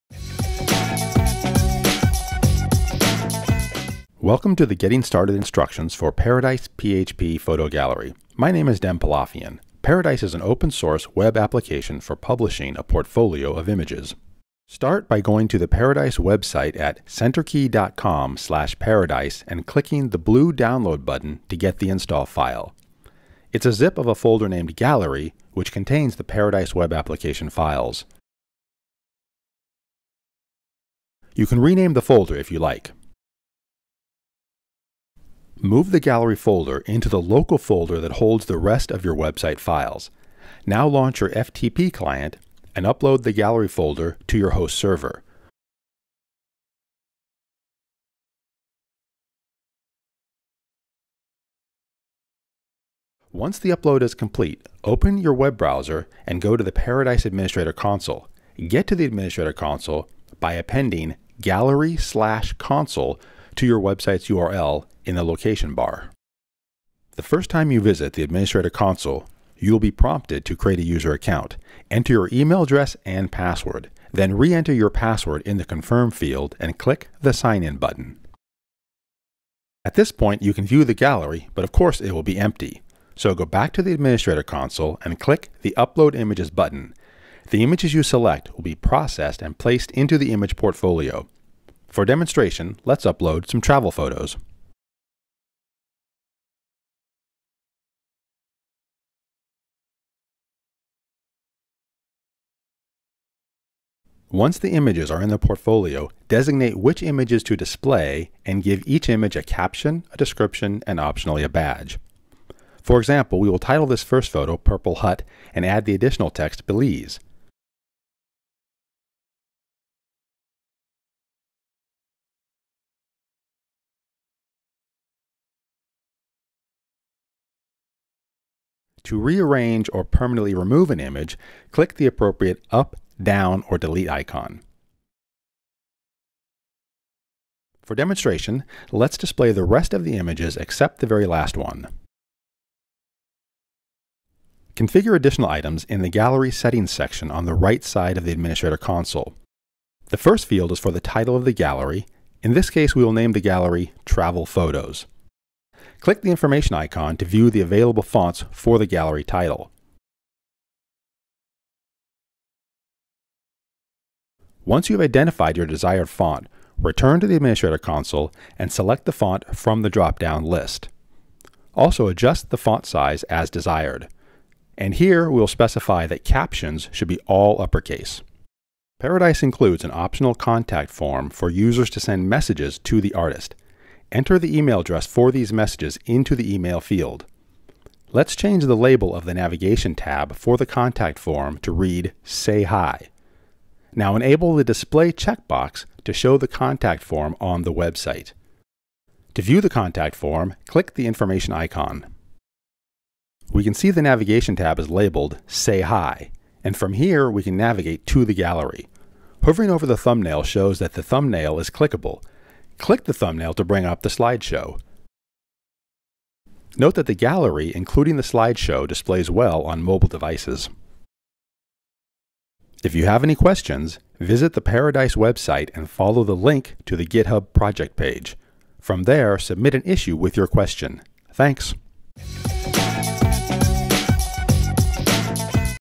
Paradise PHP Photo Gallery (Open Source) -- Getting Started
Video tutorial transcript:

1) Welcome to the Getting Started instructions for Paradise PHP Photo Gallery.  My name is Dem Pilafian.  Paradise is an open source web application for publishing a portfolio of images.

2) Start by going to the Paradise and clicking the blue download button to get the install file.  It's a zip of a folder named "gallery", which contains the Paradise web application files.  You can rename the folder if you like.

3) Move the "gallery" folder into the local folder that holds the rest of your website files.  Now launch your FTP client and upload the "gallery" folder to your host server.  Once the upload is complete, open your web browser and go to the Paradise Administrator Console.  Get to the Administrator Console by appending "gallery/console" to your website's URL in the location bar.

4) The first time you visit the Administrator Console, you will be prompted to create a user account.  Then re-enter your password in the "Confirm" field, and click the "Sign in" button.

5) At this point you can view the gallery, but of course, it will be empty.  So, go back to the Administrator Console and click the "Upload images" button.  The images you select will be processed and placed into the image portfolio.  For demonstration, let's upload some travel photos.

6) Once the images are in the portfolio, designate which images to display and give each image a caption, a description, and optionally a badge.  For example, we will title the first photo "Purple Hut" and add the additional text "Belize".  To rearrange or permanently remove an image, click the appropriate "up", "down", or "delete" icon.  For demonstration, let's display the rest of the images except the very last one.

7) Configure additional items in the Gallery Settings section on the right side of the Administrator Console.  The first field is for the title of the gallery.  In this case, we will name the gallery "Travel Photos".  Click the information icon to view the available fonts for the gallery title.  Once you have identified your desired font, return to the Administrator Console and select the font from the drop-down list.  Also, adjust the font size as desired.  And here we will specify that captions should be all uppercase.

8) Paradise includes an optional contact form for users to send messages to the artist.  Let's change the label of the navigation tab for the contact form to read "Say hi!".  Now enable the “display” checkbox to show the contact form on the website.  To view the contact form, click the information icon.

9) We can see the navigation tab is labeled “Say hi!”, and from here we can navigate to the gallery.  Hovering over a thumbnail shows that the thumbnail is clickable.  Click a thumbnail to bring up the slideshow.  Note that the gallery, including the slideshow, displays well on mobile devices.

10) If you have any questions, visit the Paradise website and follow the link to the GitHub project page.  From there, submit an issue with your question.  Thanks.